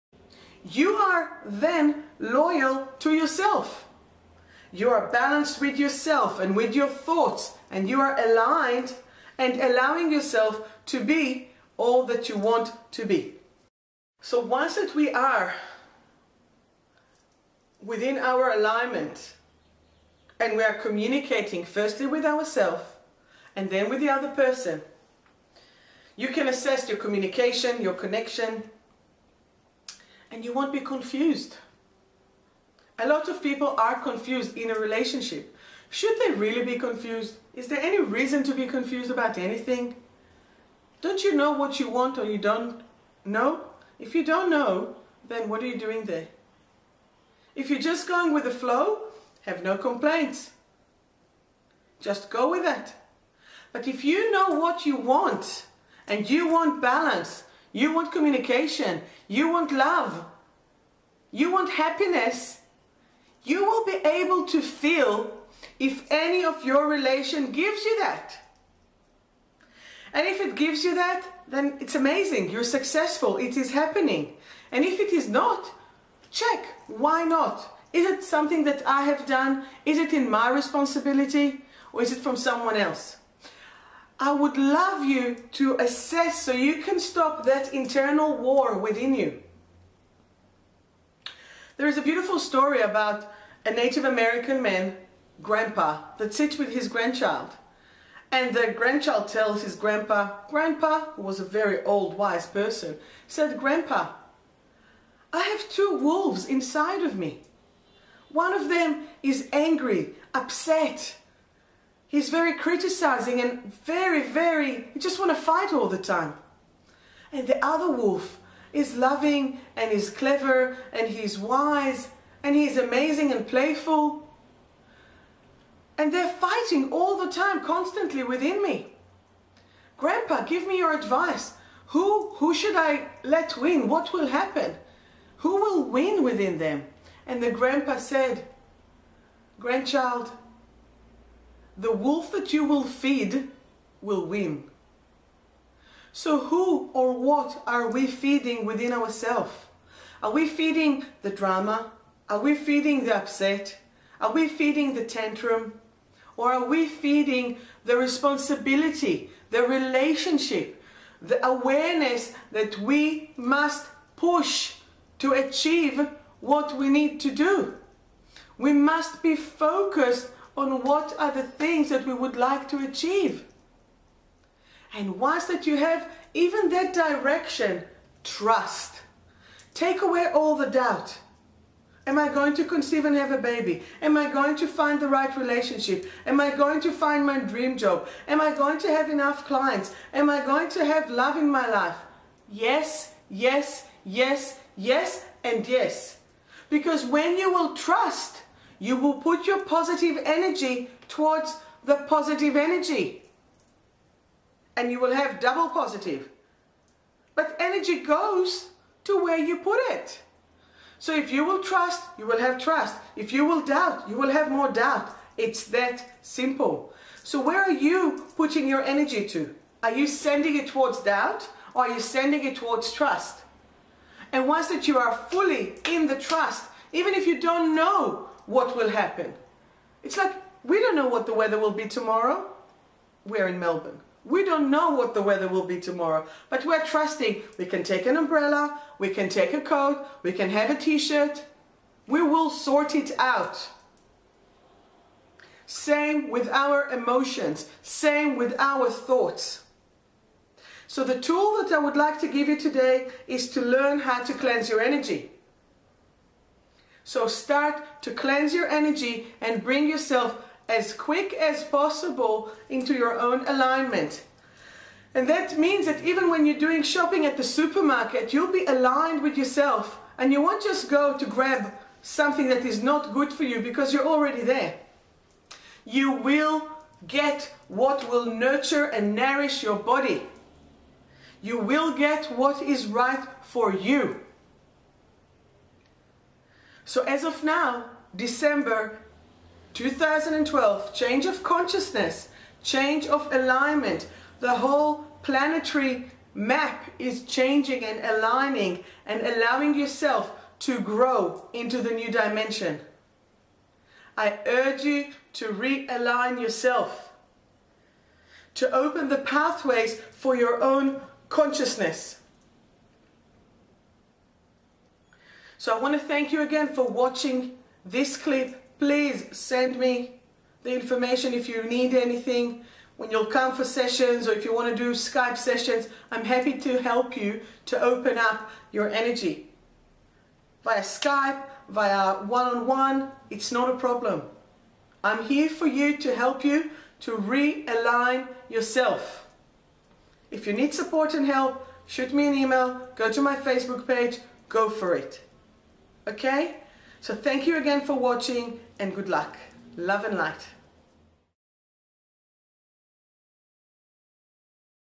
The War Within with Sharon Tal P2
At the time of this video it is December 2012, an extreme energetic time period. The war we feel within ourselves is reflecting the war expressed in our outer world.

It is the fight between the light and the dark, love and fear, what we want and what we have to face/experience. At this time relationships are being tested or dissolving, and what is paramount is that we remain truthful and faithful to ourselves

This video covers the "C" words in relationships

Communication - Do you communicate? Can you communicate? in a way that helps the other person to understand the message you wish to express to them?

Connection - where is your connection or the source of your connection with the other person?

Chakras - Are you aligned within yourself? Are there any blockages in your energy?

Control - Do you control the connection, the communication? Are you being controlled? Is it balanced?

Catalyst - In relationships one person will often be the catalyst to the other. One side will see the potential in the other, but is the other side ready for it?

Choice - Decide if you want to communicate, Do you want to be in this relationship, Do you/ can you accept it for what it is?